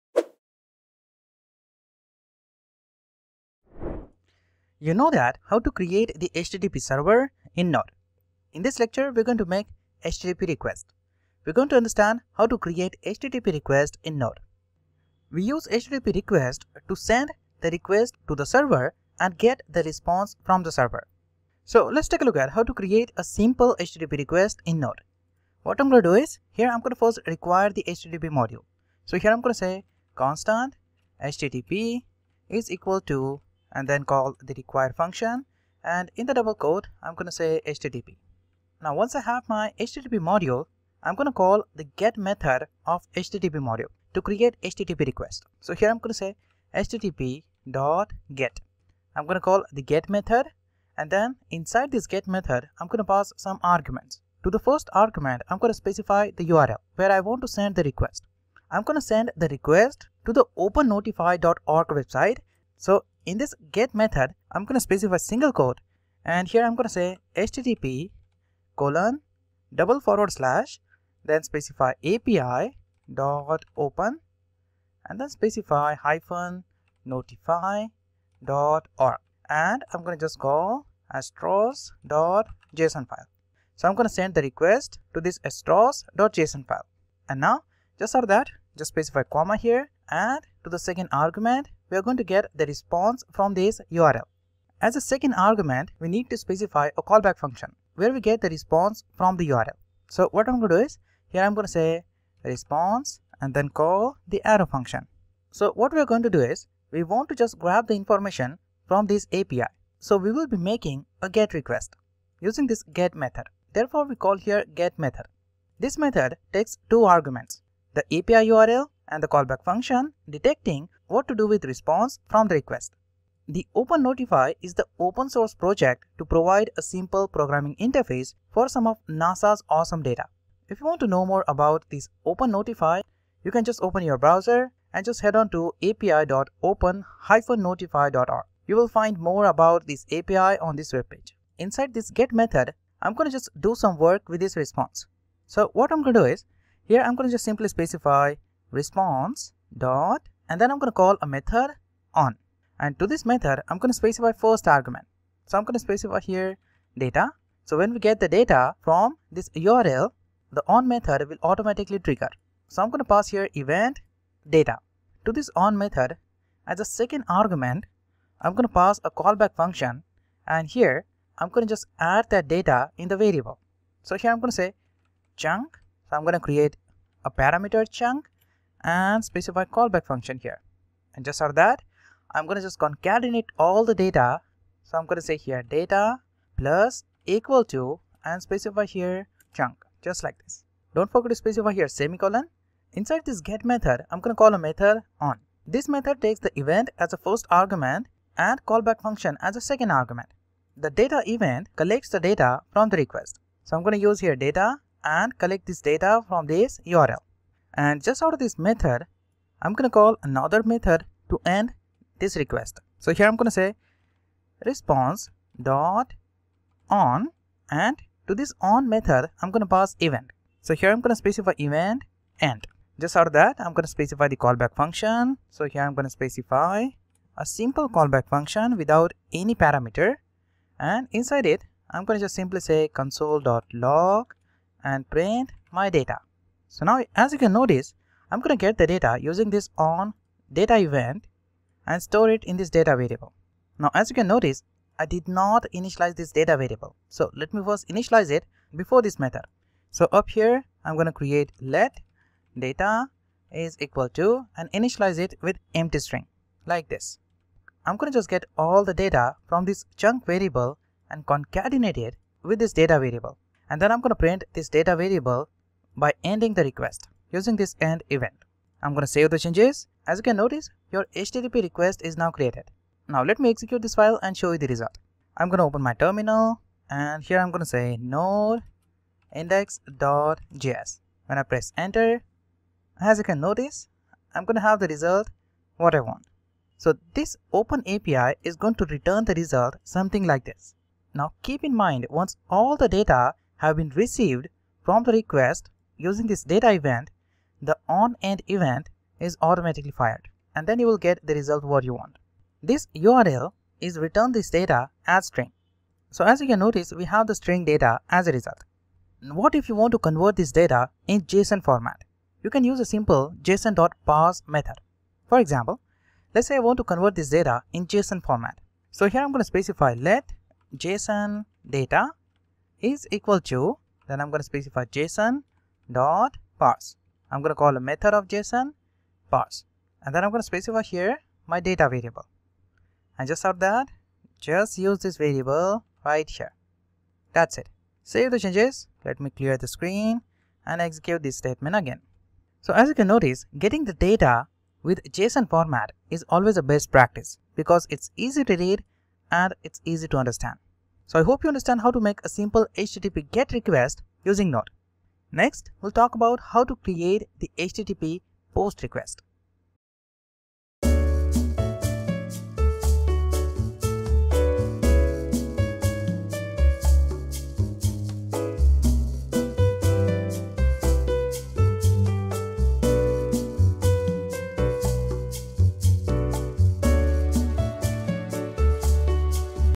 Making HTTP Request - Node For Beginners - 15
In this lecture, we are going to learn how to make an HTTP request using a node. In the previous video, we learn how to create an HTTP server.
This lecture will teach you how to make an HTTP requests using node.